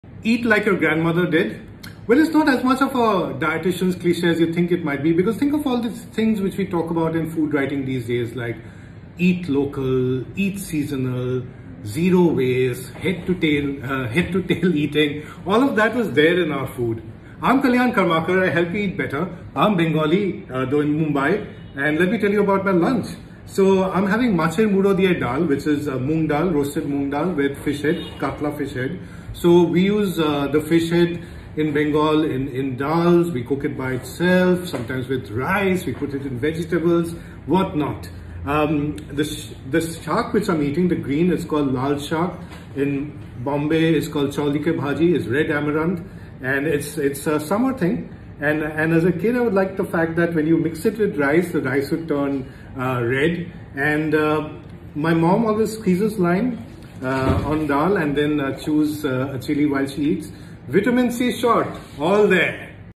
Eat like your grandma to eatbetter
A message from yesterday’s lunch. Want to eatbetter? Check what grandma did. Be sensible and factor in our reduced activity levels compared to her and so on and imbibe her principles instead. This is from Bengal. Tell me about some of your grandma’s eating habits which makes a lot of sense to you.

Stat warning: Diets should be customised and please check with your doc on what works for you.